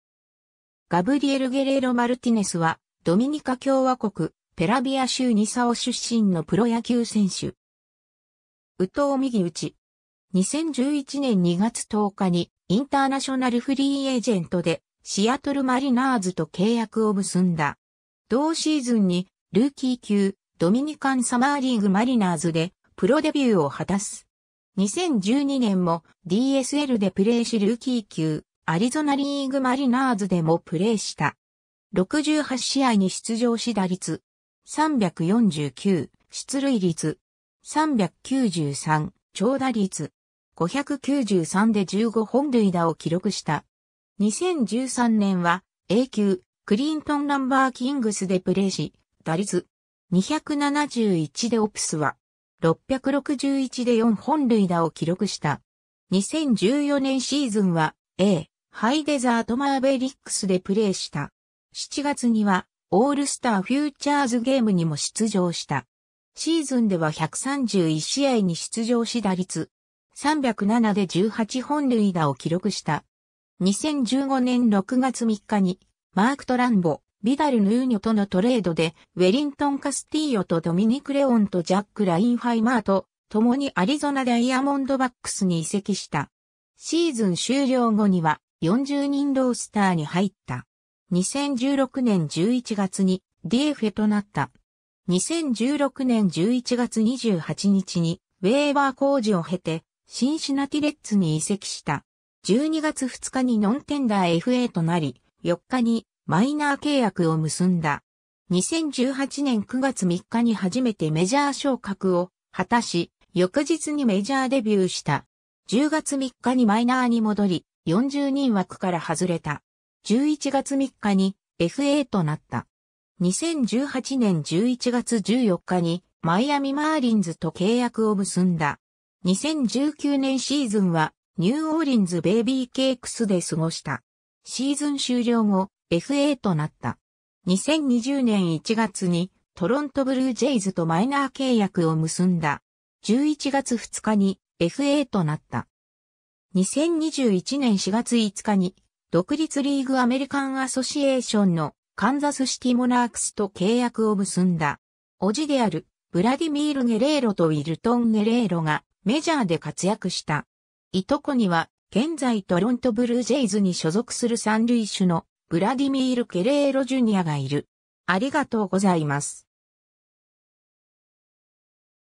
ガブリエル・ゲレーロ
ガブリエル・ゲレーロ・マルティネス（Gabriel Guerrero Martinez、1993年12月11日 - ）は、ドミニカ共和国ペラビア州ニサオ出身のプロ野球選手（外野手）。
右投右打。
 2011年2月10日にインターナショナル・フリーエージェントでシアトル・マリナーズと契約を結んだ。
同シーズンにルーキー級ドミニカン・サマーリーグ・マリナーズ(DSL)でプロデビューを果たす。
 2012年もDSLでプレーしルーキー級アリゾナリーグ・マリナーズでもプレーした。
68試合に出場し打率.349、出塁率.393、長打率.593で15本塁打を記録した。
 2013年はA級クリントン・ランバーキングスでプレーし、打率.271でOPSは.661で4本塁打を記録した。
 2014年シーズンはA+ハイデザート・マーベリックスでプレーした。
7月にはオールスター・フューチャーズゲームにも出場した。
シーズンでは131試合に出場し打率.307で18本塁打を記録した。
 2015年6月3日にマーク・トランボ、ビダル・ヌーニョとのトレードでウェリントン・カスティーヨとドミニク・レオンとジャック・ラインハイマーと共にアリゾナ・ダイヤモンドバックスに移籍した。
シーズン終了後には、40人ロースターに入った。
 2016年11月にDFAとなった。
 2016年11月28日にウェイバー公示を経てシンシナティ・レッズに移籍した。
12月2日にノンテンダーFAとなり、4日にマイナー契約を結んだ。
 2018年9月3日に初めてメジャー昇格を果たし、翌日にメジャーデビューした。
10月3日にマイナーに戻り、40人枠から外れた。
11月3日にFAとなった。
 2018年11月14日にマイアミ・マーリンズと契約を結んだ。
 2019年シーズンはニューオーリンズ・ベイビーケークスで過ごした。
シーズン終了後、FAとなった。
 2020年1月にトロント・ブルージェイズとマイナー契約を結んだ。
11月2日にFAとなった。
 2021年4月5日に独立リーグ・アメリカン・アソシエーションのカンザスシティ・モナークスと契約を結んだ。
 叔父である、ブラディミール・ゲレーロとウィルトン・ゲレーロがメジャーで活躍した。
いとこには、現在トロント・ブルージェイズに所属する三塁手のブラディミール・ゲレーロ・ジュニアがいる。